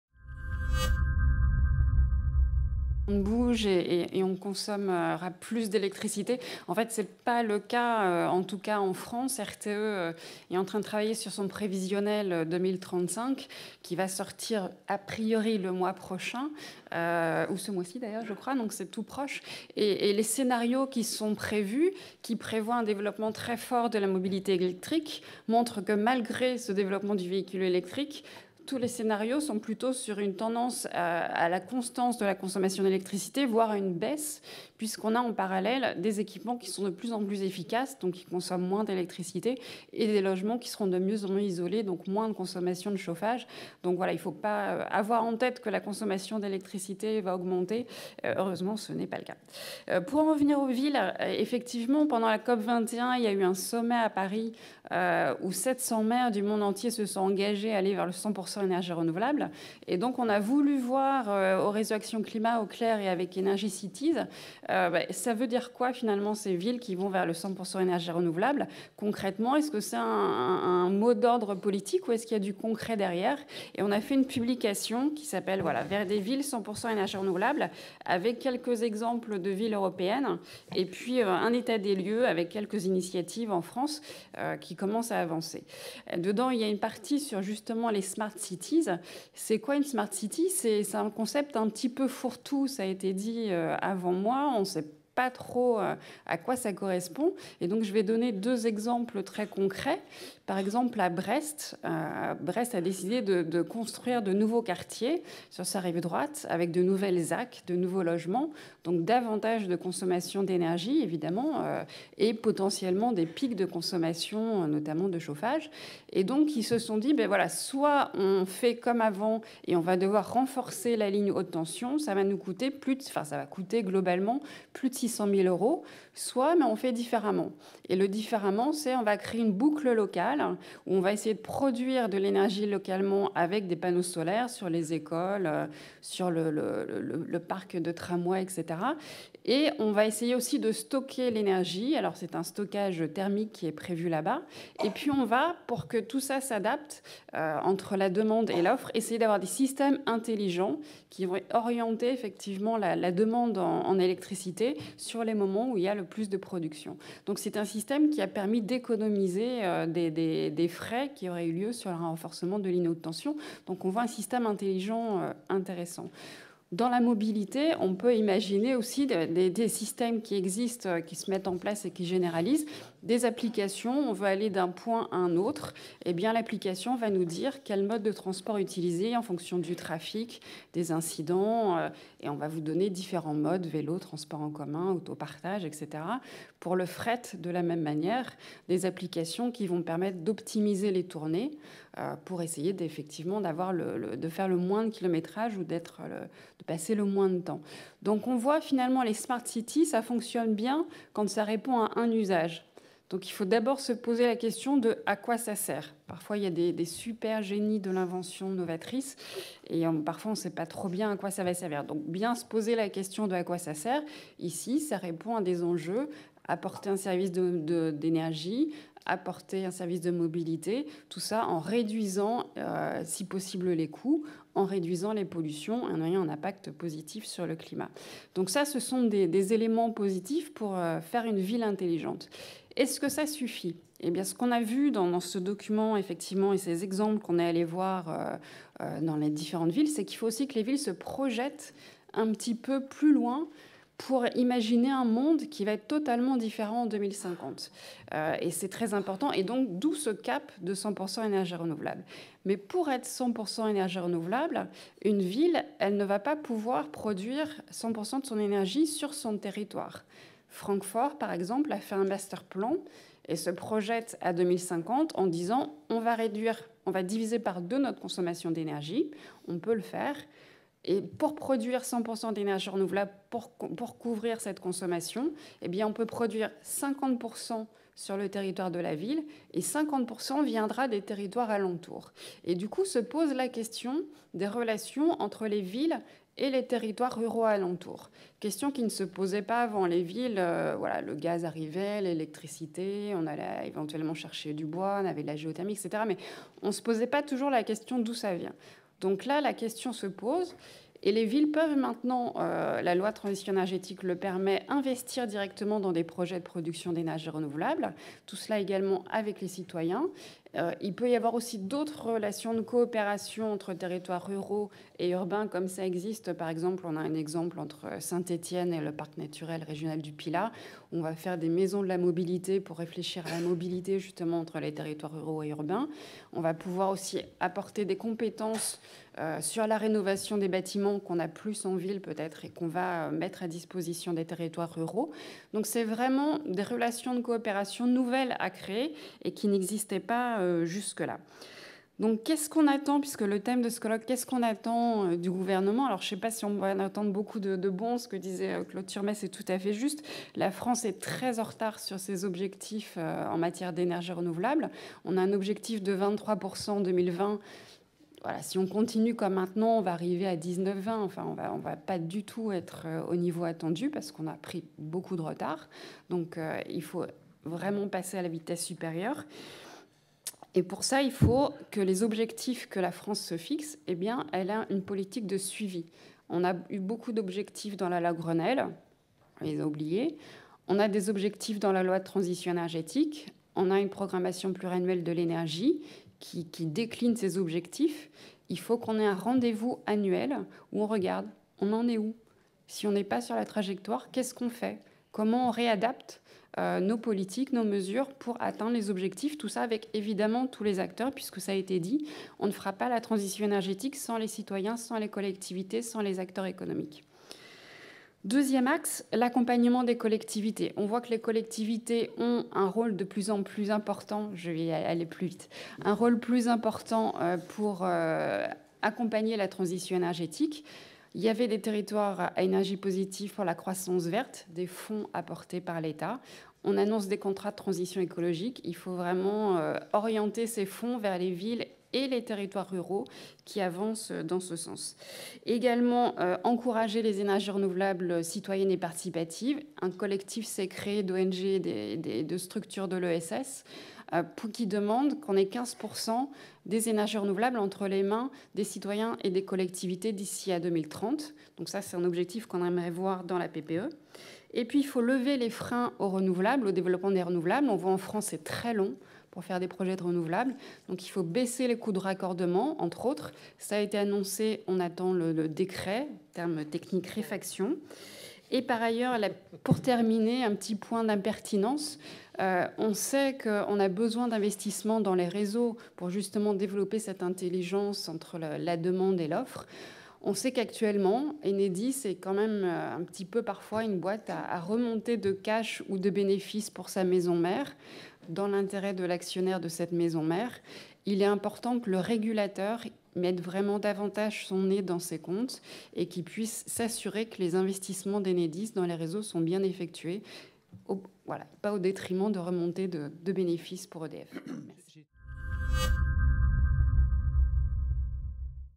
Opening statement: Anne Bringault
Anne Bringault est membre du CLER -  Réseau Action Climat France.